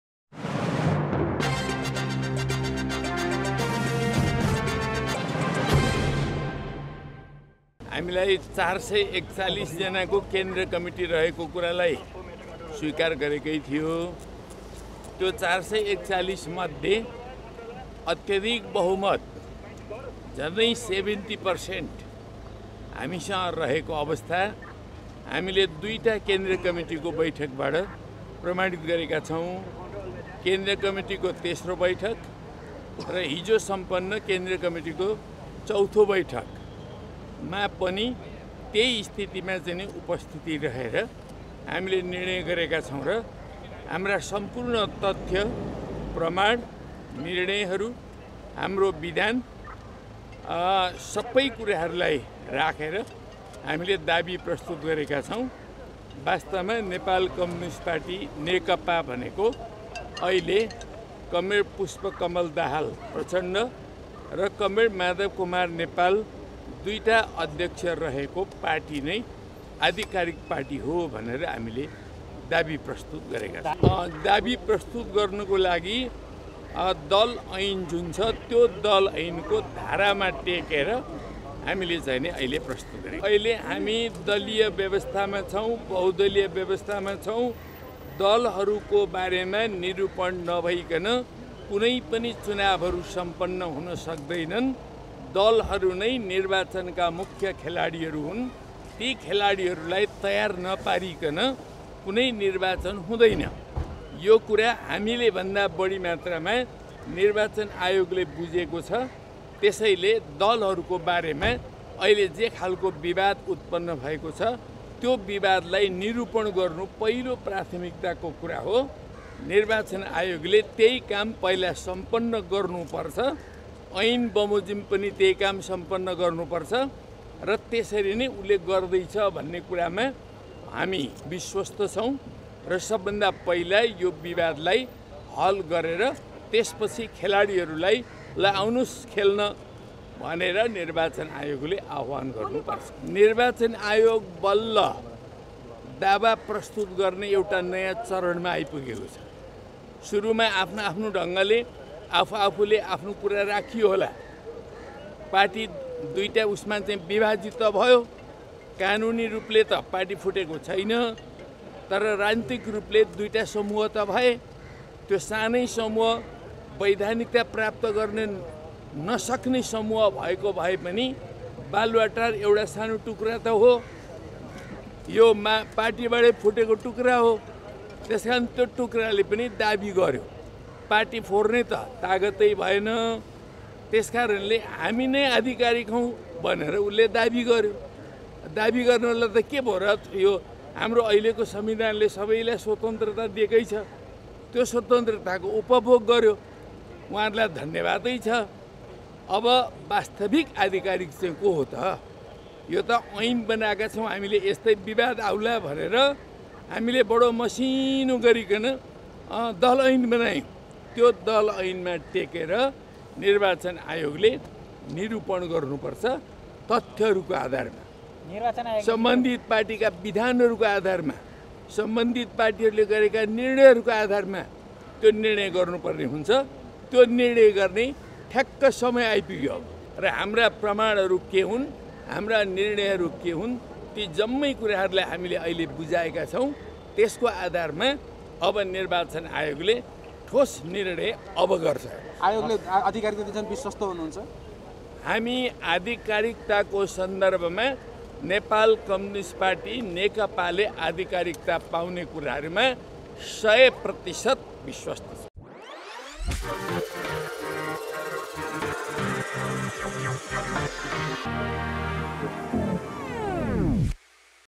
पार्टीको आधिकारिकता हामीले पाउने कुरामा विश्वस्त छौं : झलनाथ खनाल
पार्टीको आधिकारिकता हामीले पाउने कुरामा विश्वस्त छौं : झलनाथ खनाल jhalnathkhanal Nekapa prachanda Madhavkumarnepal # RatopatiTV